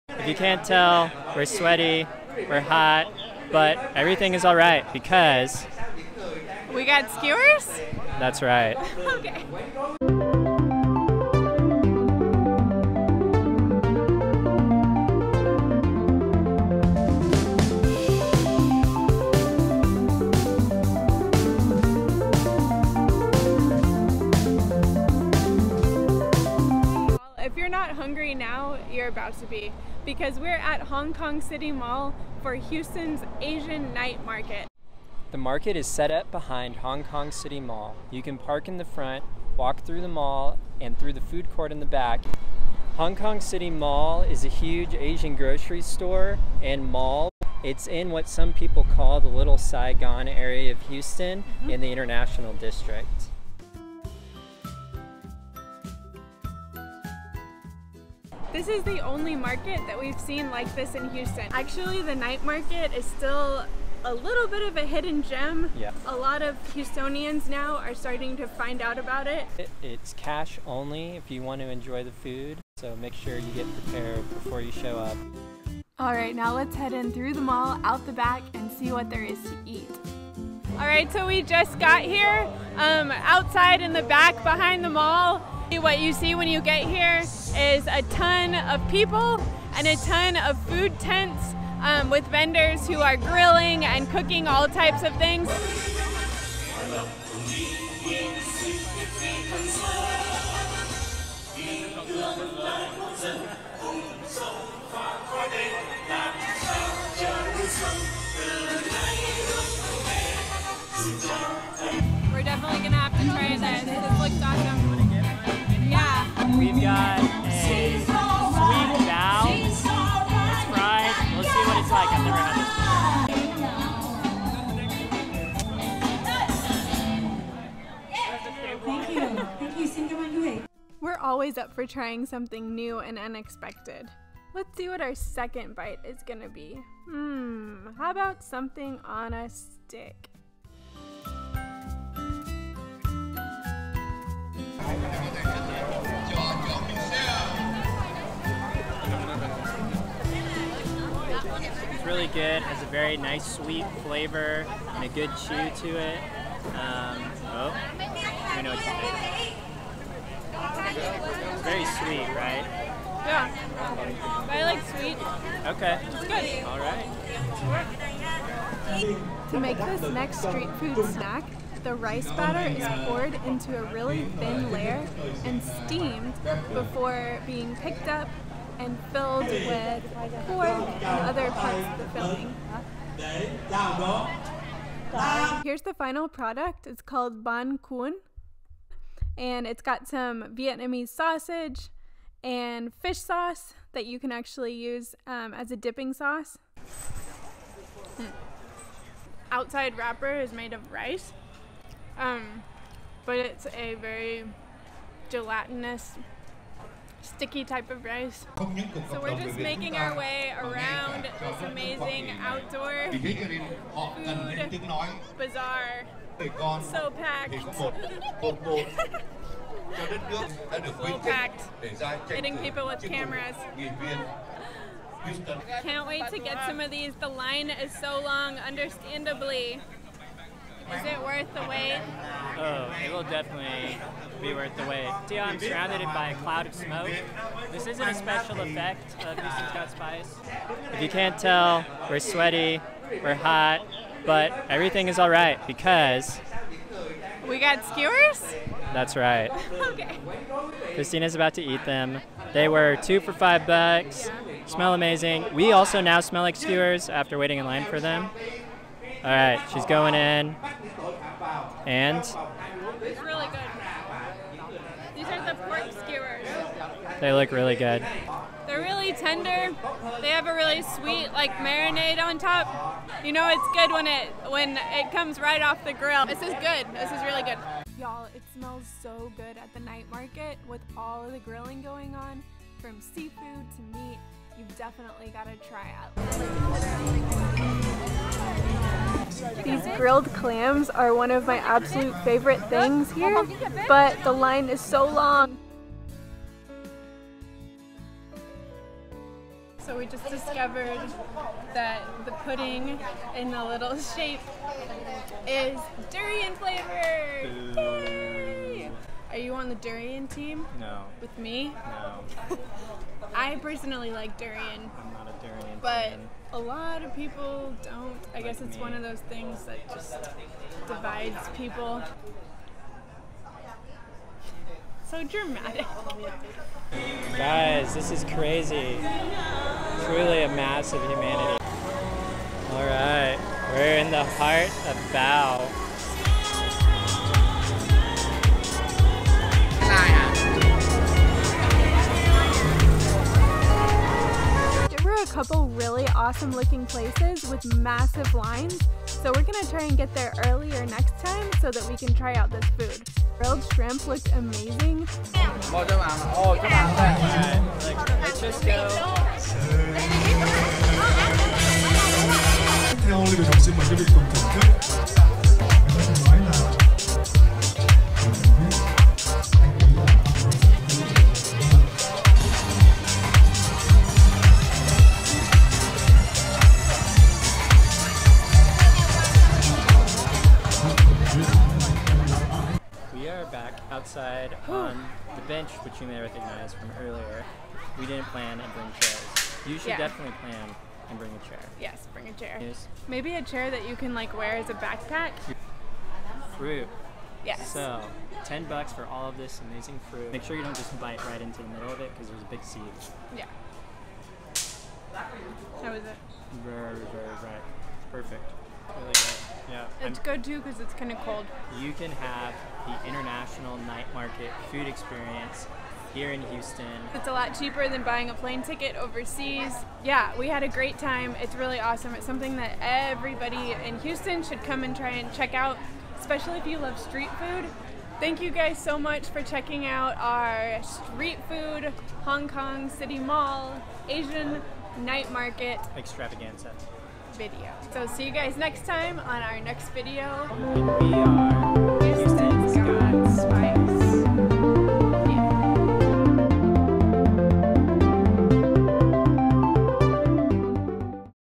Houston's Asian Night Market!
Hong Kong City Mall is home to Houston's one and only Asian Night Market! This is the best place in Houston to get a true street food experience! From grilled seafood to pork skewers, to fresh fruit and boba, this Night Market is a mini foodie heaven!

Where: Hong Kong City Mall (Bellaire Blvd)
When: Every couple months; find "Asian Night Market Cho Dem" on Facebook for the next dates to be announced.
Time: 6PM - 10PM Friday - Sunday
Notes: Bring cash, (cash only!) and a portable chair! Come early because it gets crowded.